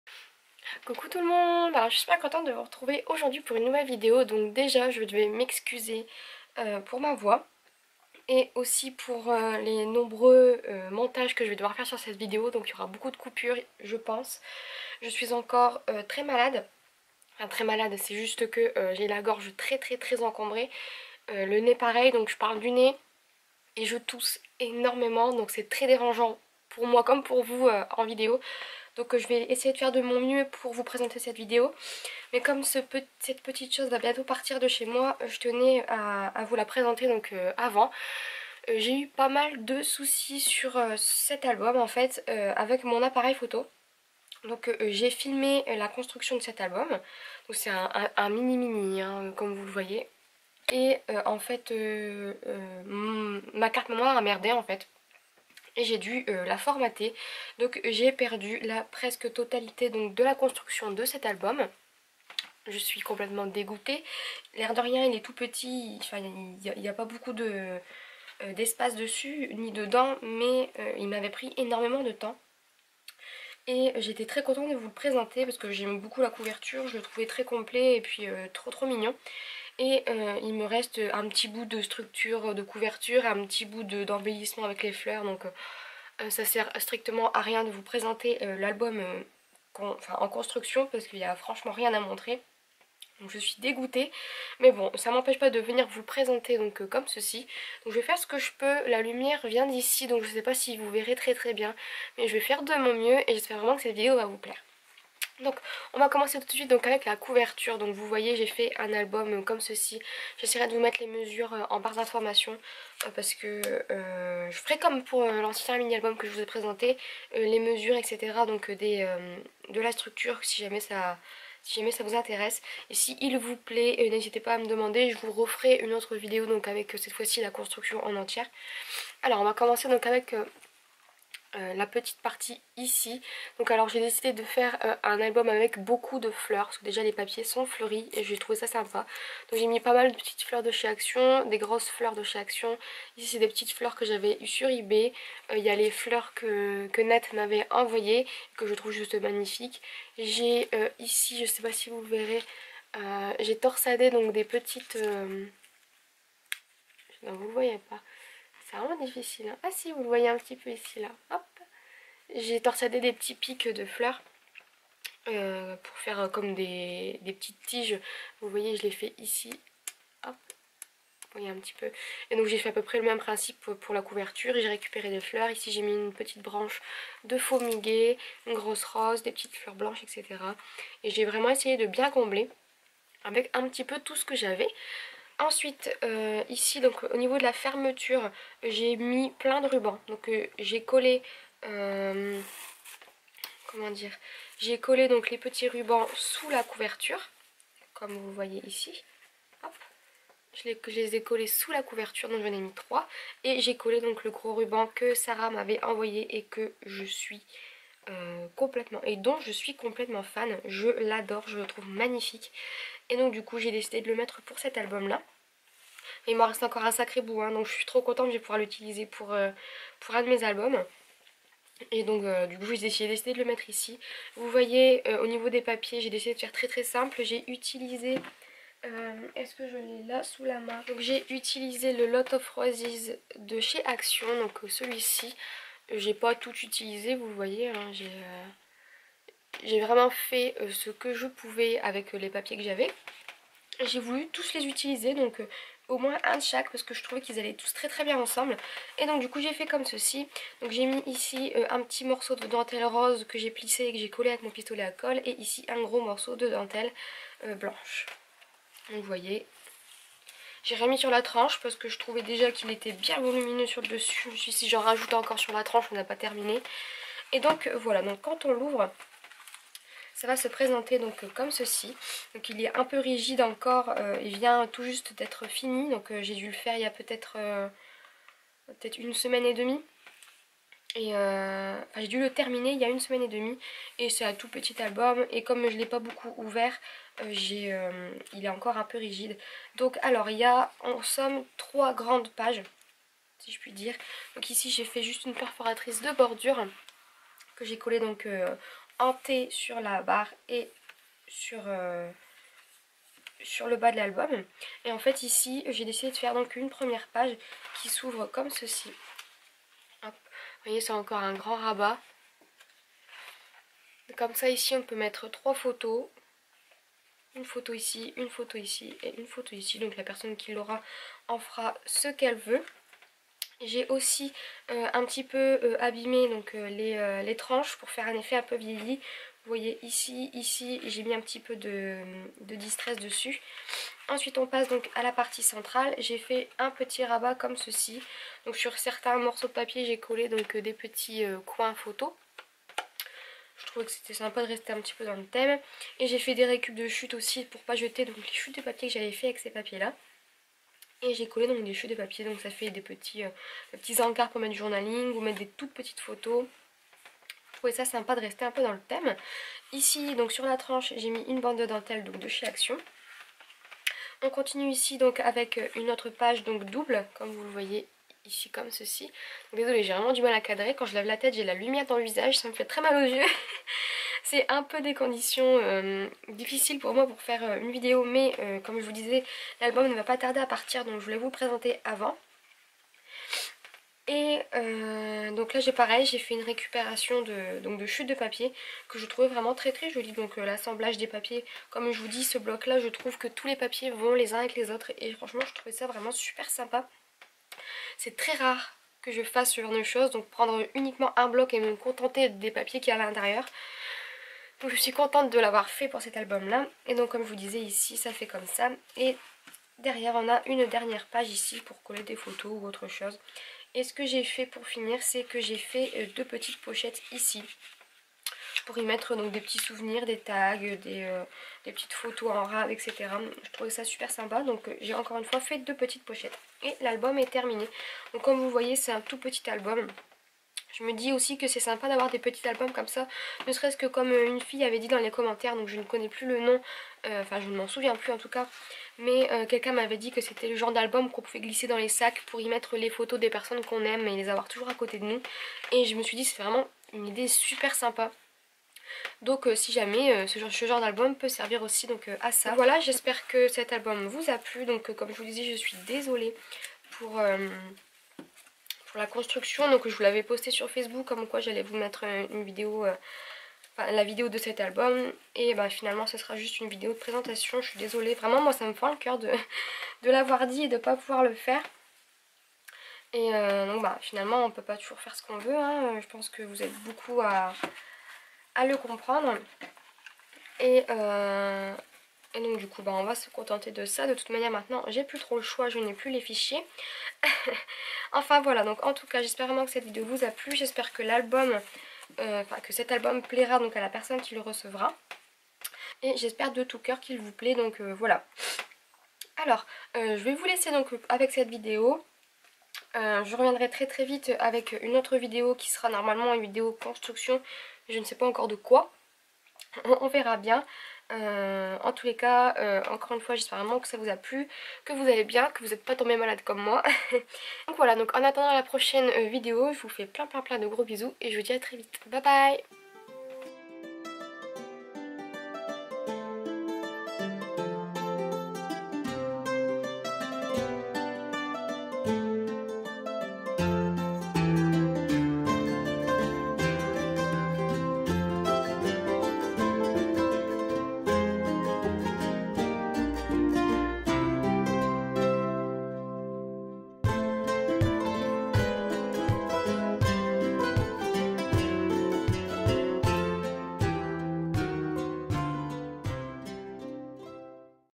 Mini Album Petit & fleuris ♡
♡ Plus d'informations sur cette vidéo ♡

♡ Dimensions de l'album :
- Structure : 16.5 x 11.5 x 2.5
- Page 1 : 16.5 x 29.5 (pliage à 8.5, 18.5, 28.5)
- Page 2 :  10 x 20.5 (pliage à 9.5, 19.5)
- Page 3 : 16.5 x 19.5 (pliage à 1, 11)
- Pochettes : 12 x 5 ( pliage à 0.5 du bord droit, gauche et bas)

♡ Produits utilisés :
- Bloc de papier Action Lots of Roses
- Fleurs diverses - Action, Noz, Cadeaux
- Rubans : Action et Cadeaux
- Dentelles : Action et Récup

Milles bisous ♥